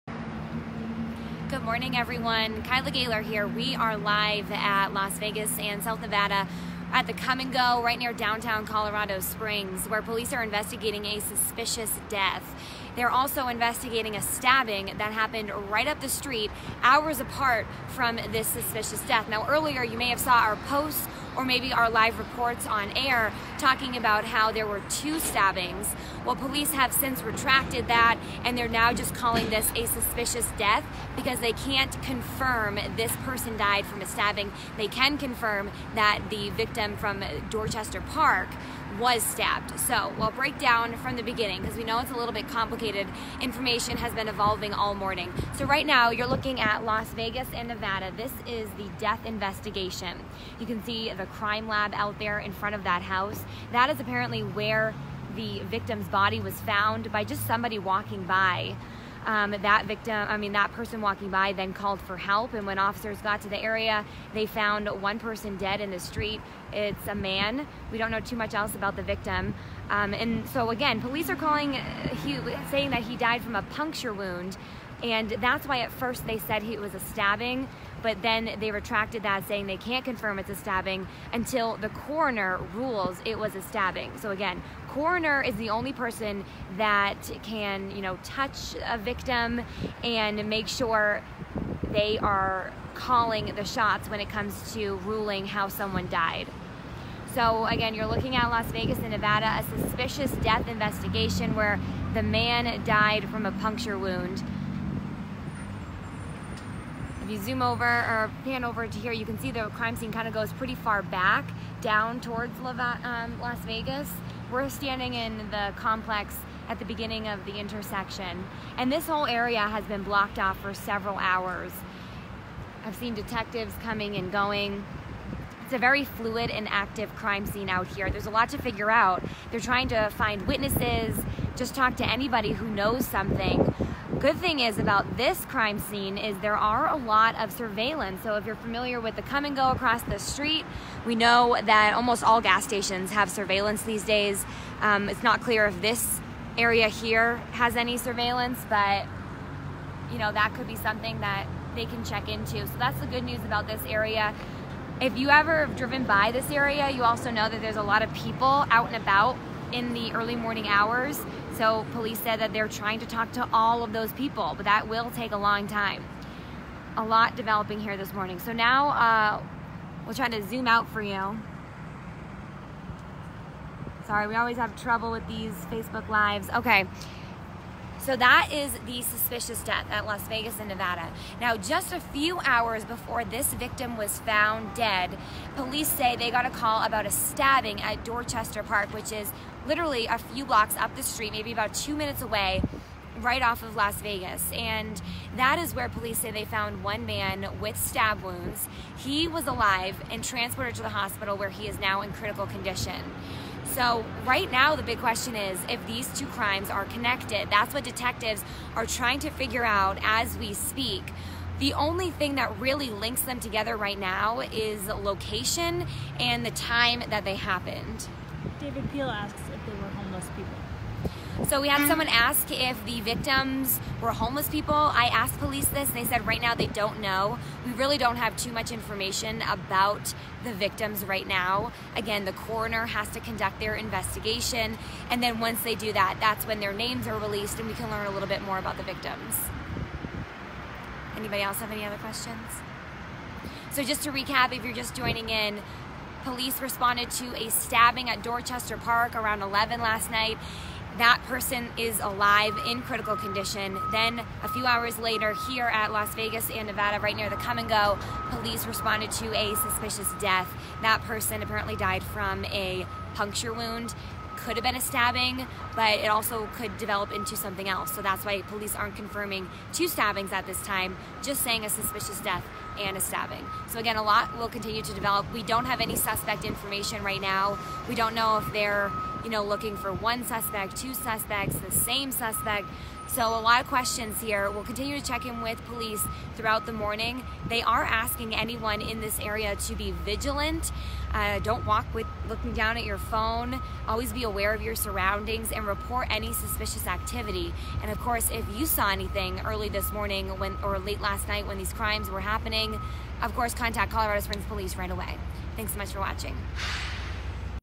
KKTV Facebook Live stabbing investigation
On-the-scene reporting of an active police investigation in Colorado Springs.  Homeless involved?